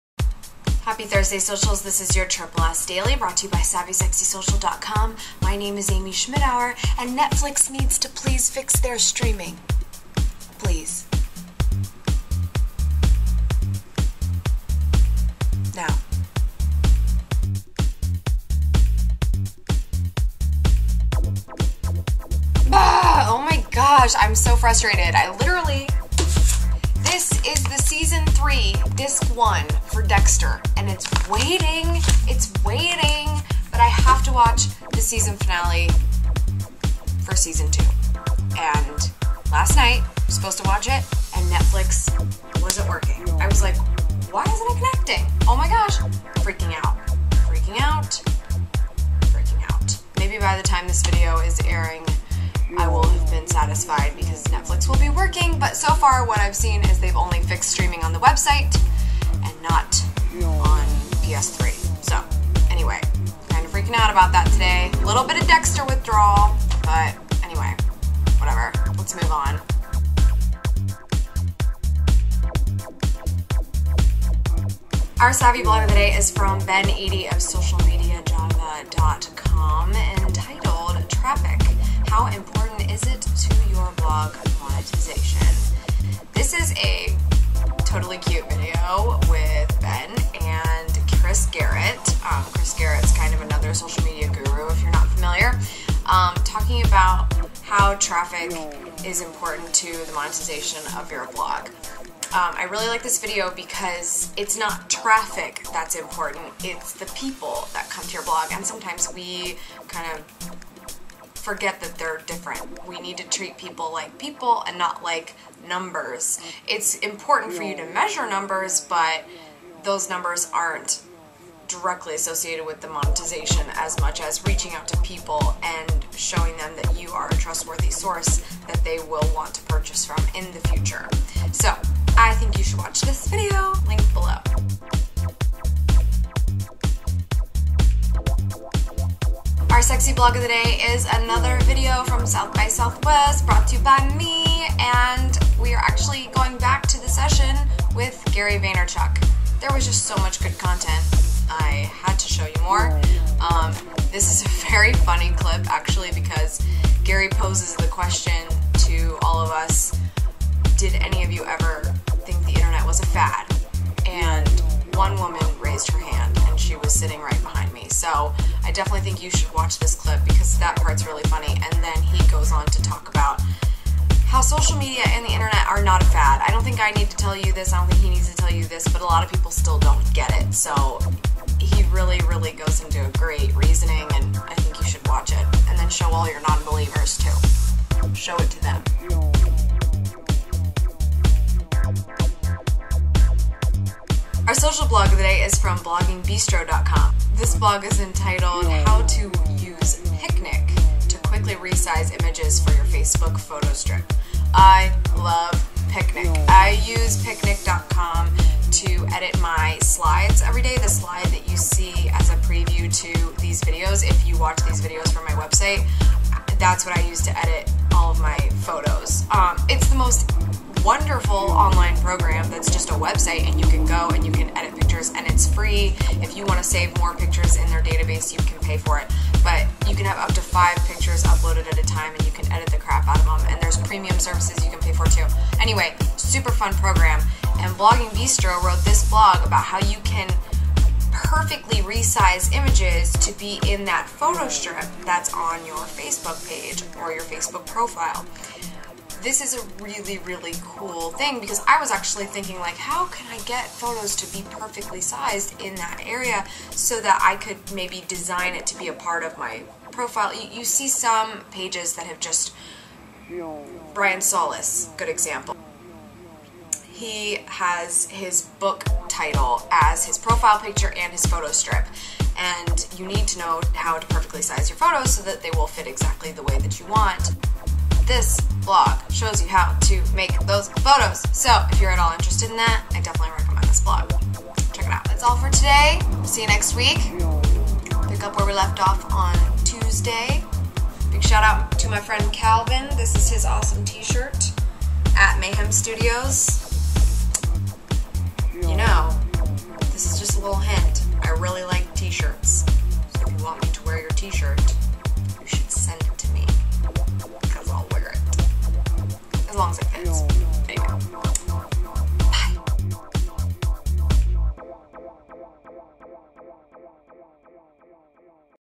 Traffic and Monetization For Your Blog. How do they relate?
Happy Thursday! Today we have the importance of people vs. traffic when it comes to monetizing your blog, another fab clip from Gary Vaynerchuk, and an awesome instruction blog on how to manipulate you Facebook photostrip to look exactly how you want!